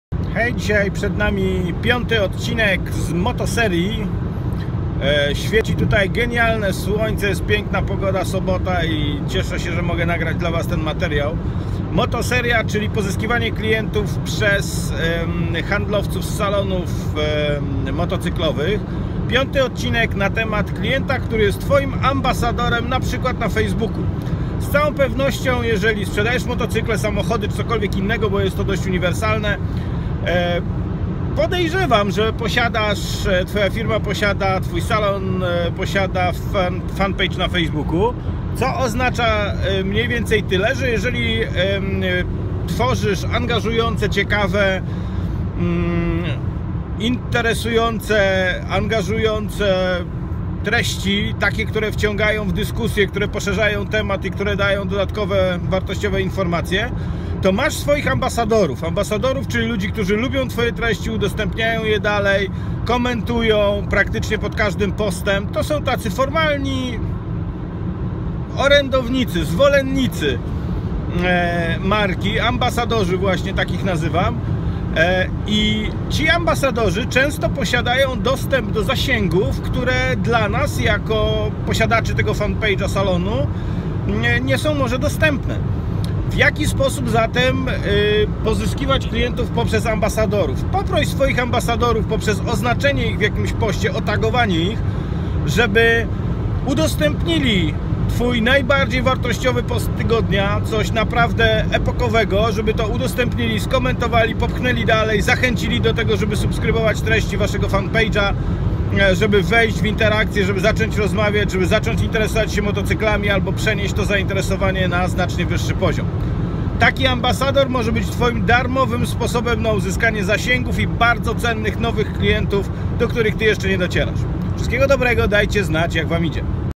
Seria moto: klient-ambasador, czyli odc. 145 z cyklu "Moje dwie minuty"
Klienci-ambasadorzy, czyli tacy, którzy z własnej inicjatywy wspierają i promują naszą markę, są jej zagorzałymi fanami i pomagają nam dotrzeć do nowych potencjalnych klientów nadają się do tego, aby ich ozłocić :).

Co jeszcze można z nimi zrobić i jak skorzystać z ich nieformalnej pomocy? Zapraszam do oglądania filmiku!